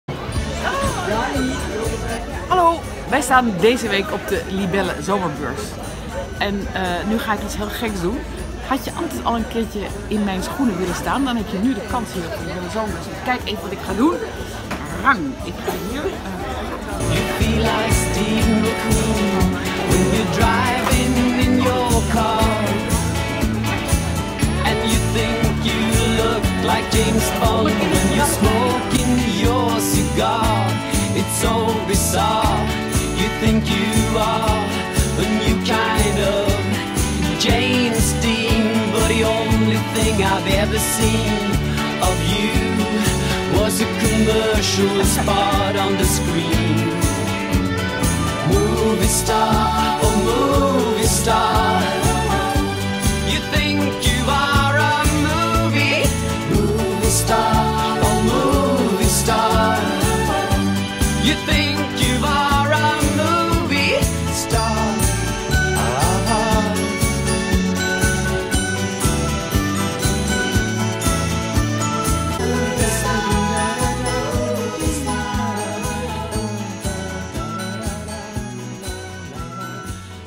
Lookalike Maroeska Metz
Heb je altijd al eens een keukenprins(es) willen zijn of in mijn schoenen willen staan, kijk dan even naar dit filmpje dat we tijdens de Libelle Zomerweek in Almere maakten. We staan er ter promotie van mijn kookboek Hoe Dan?! nog tot en met woensdag 29 mei op de beurs, standnummer 1019.